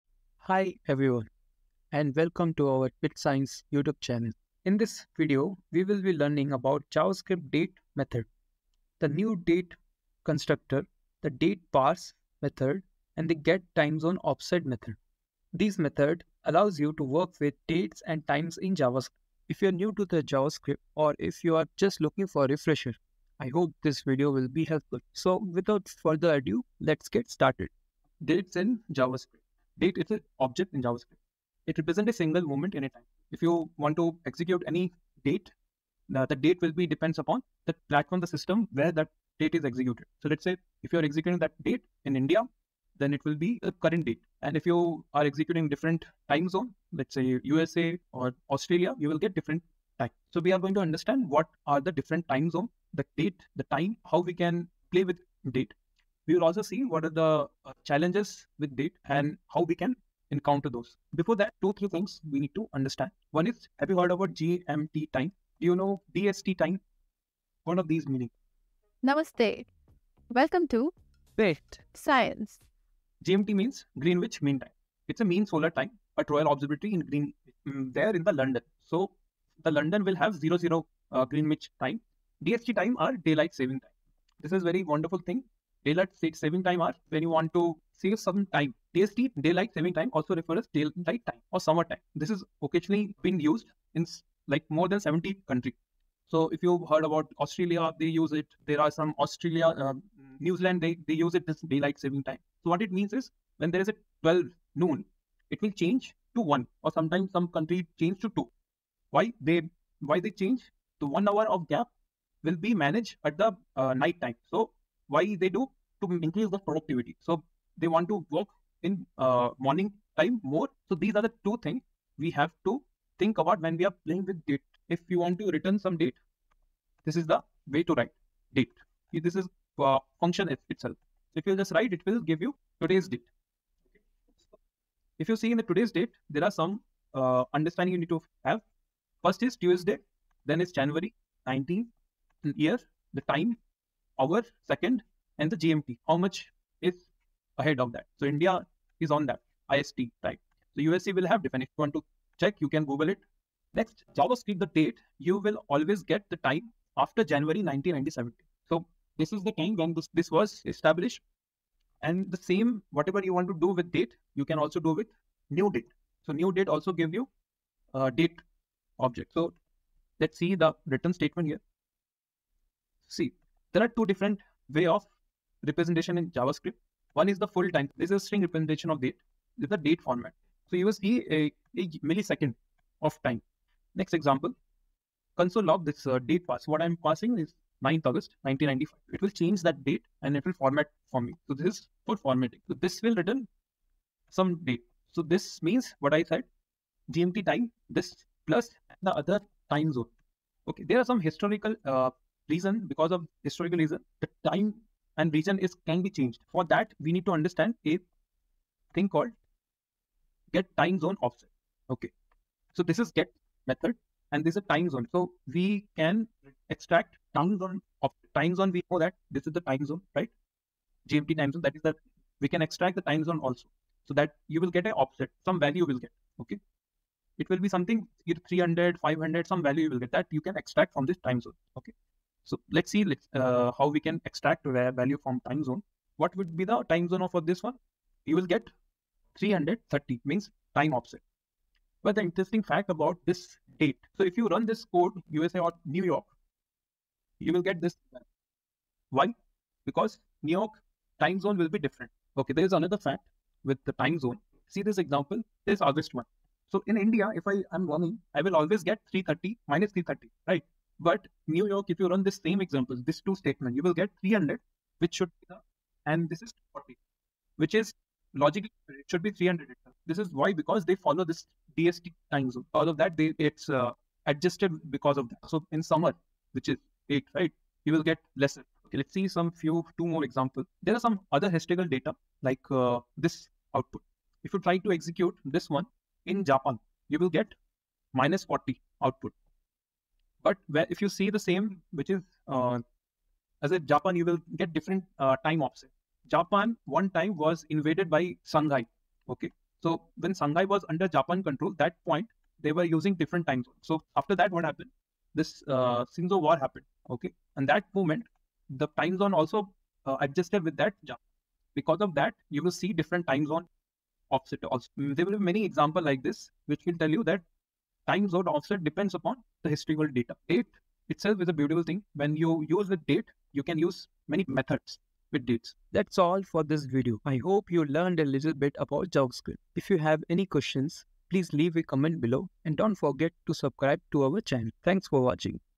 JavaScript date Methods: Learn how to work with dates and times
0:28 Date( ) method
2:41 new Date( )
3:06 Date.parse( )
3:26 getTimezoneOffset( )